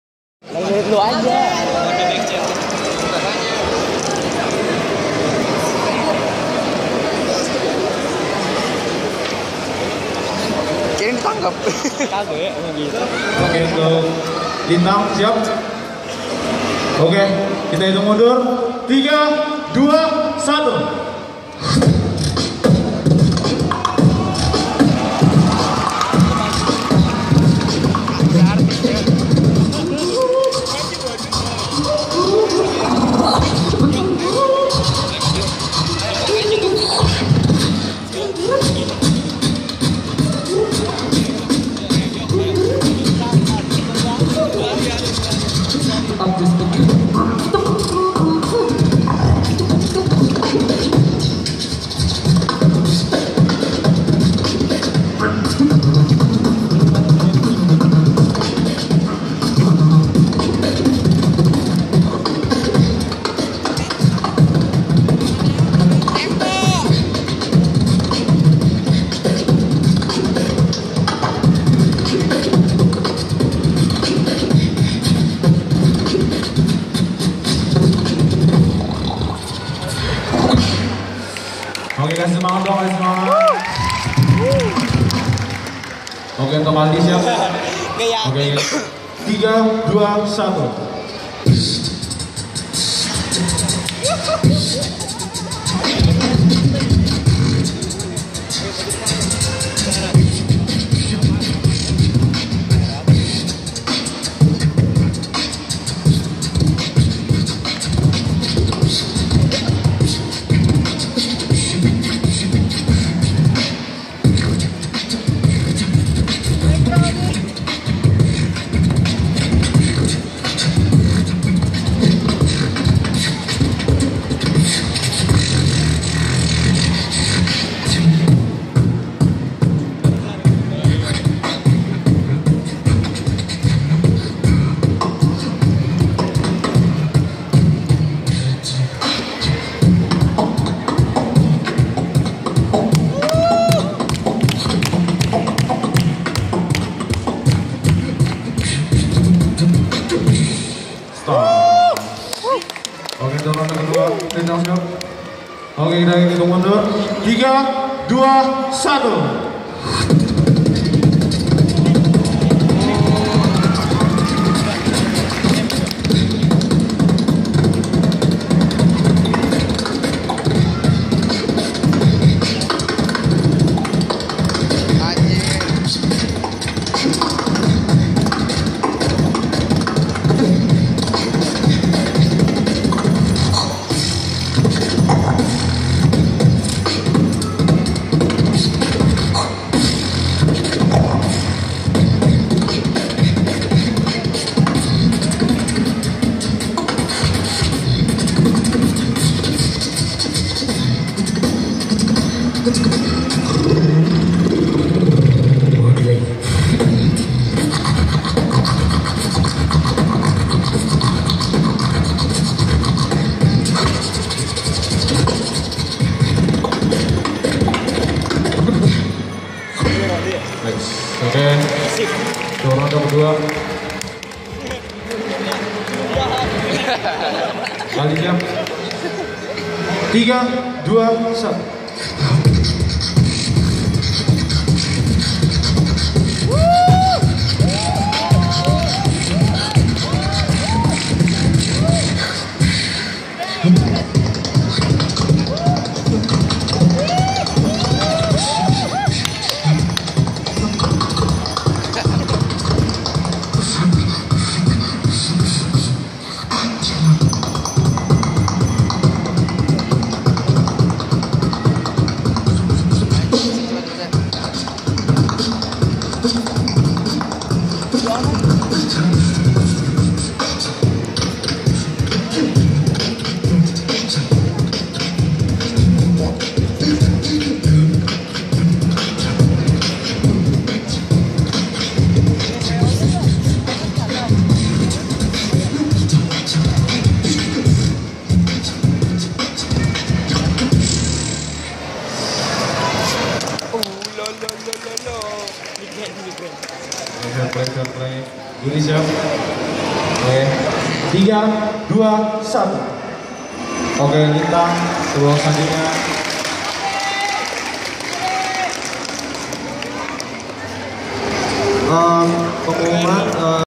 Lintang vs Aldi | Beatbox | Reincarnation Anapolo
Sanggar Kipas SMAN 2 BEKASI
Proudly Present
4th Reincarnation Anapolo
At Blu Plaza, Bekasi, Indonesia